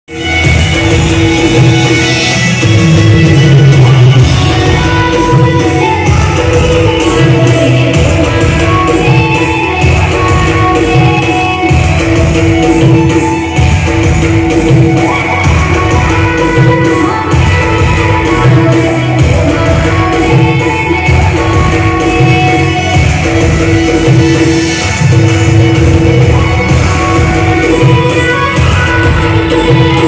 College girls dance on Gujarati song.
Video from ꧁དℭ℟Åℤ¥༒₭ÏḼḼ℥℟ཌ꧂◤꧁☆☬vikas